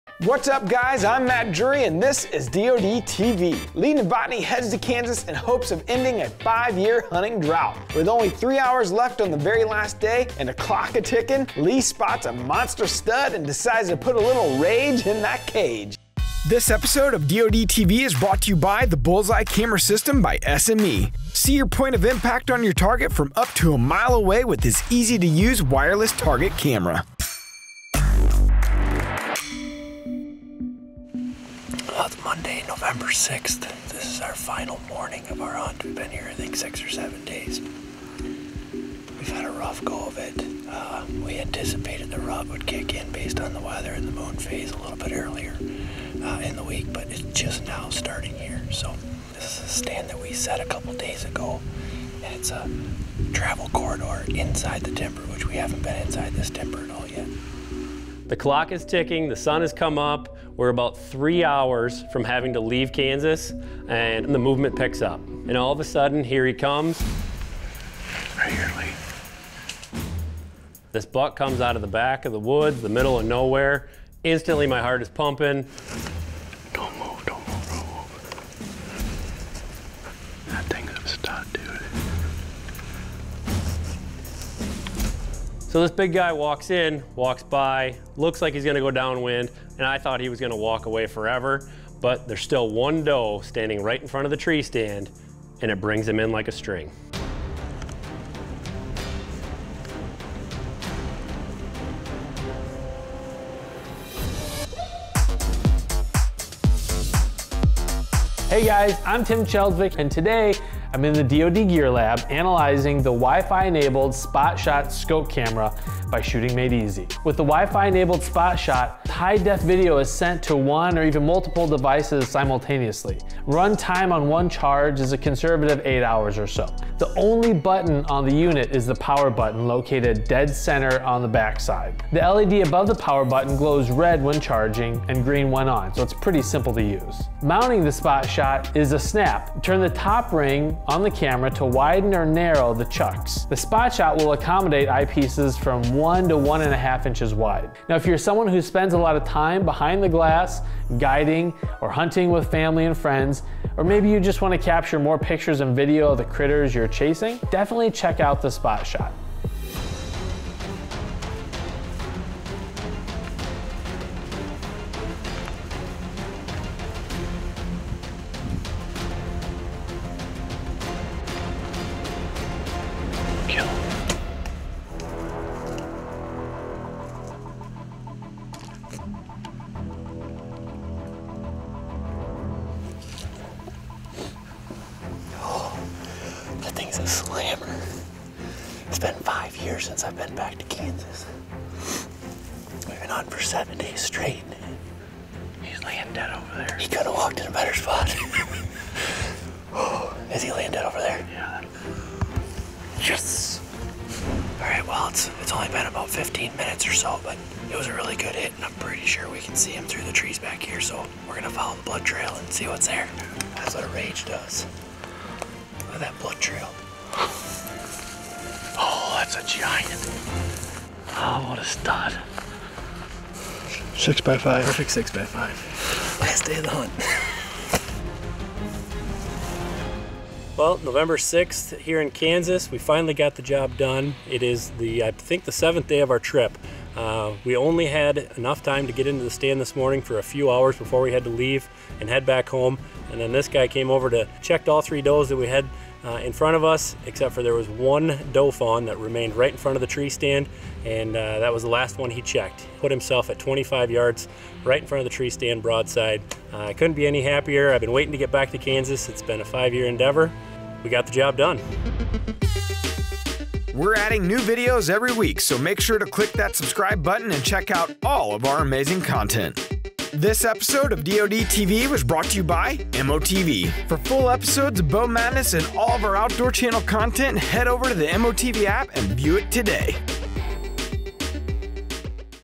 LAST DAY STUD! Can Lee Make it Happen in Only THREE HOURS!?
Time is ticking down for Lee Novotny to nab a shooter! With just three hours left on the last day, can he bring home the bacon!?

To register for the farm giveaway, and to access all the same features you love about DeerCast, now on your laptop, head on over to: